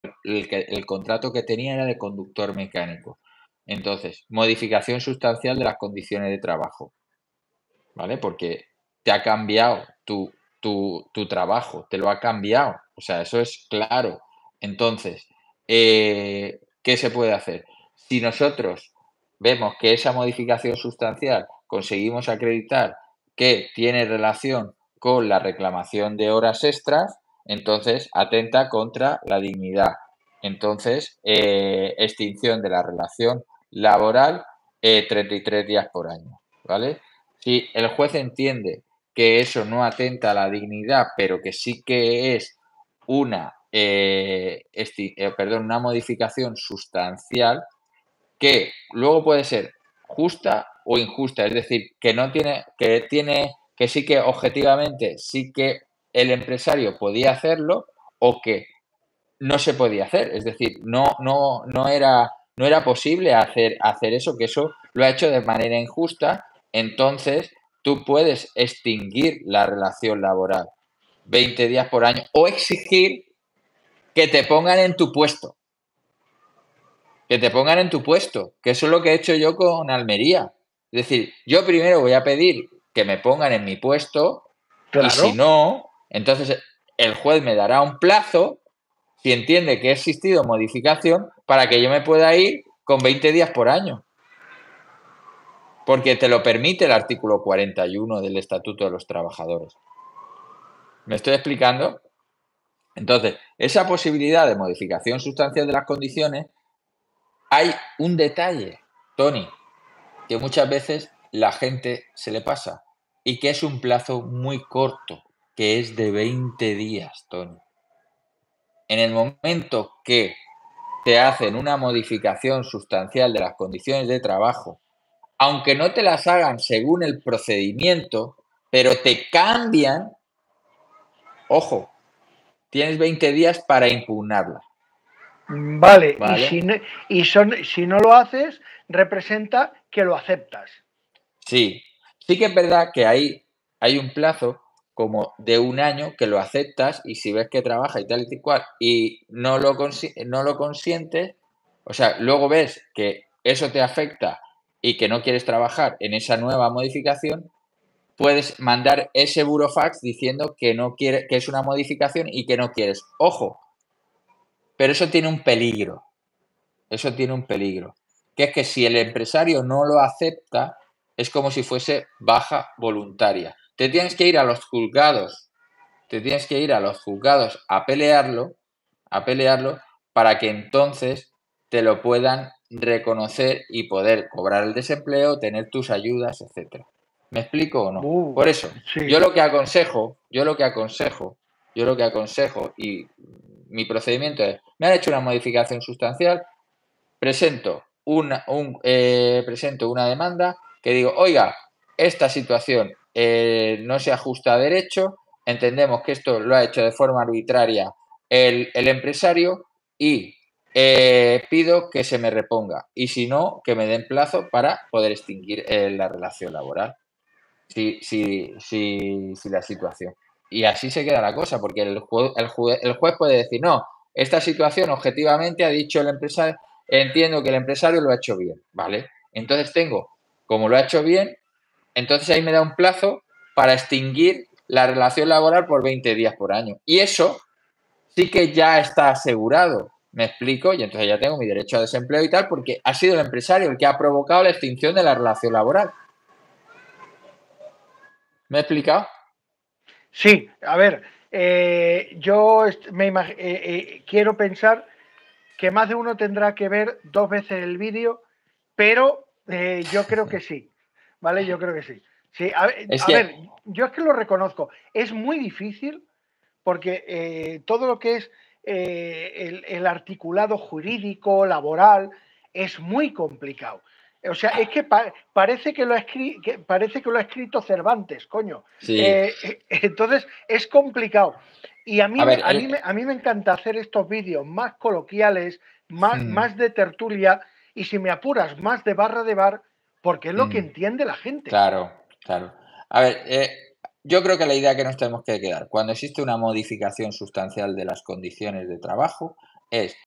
DEMANDA por Modificación sustancial de las condiciones de trabajo; ojo! con los plazos para reclamar
no te pierdas los programas completos con nuestras entrevistas de José Luis Sánchez de Sánchez cuesta abogados
en este caso tocamos en el directo completo, temas como el poder detectar el acoso laboral y la importancia de la cronología de Google Maps a nivel judicial

aquí tenéis un momento destacado de este programa tan bueno, donde José Luis Sánchez desde Sánchez cuesta abogados, nos habla de los cambios sustanciales en el puesto de trabajo y muy importante el saber los plazos para impugnarlo

¿Quieres salir en el canal?

Donaciones para materiales y desplazamientos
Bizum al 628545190
❤️ muchísimas gracias ❤️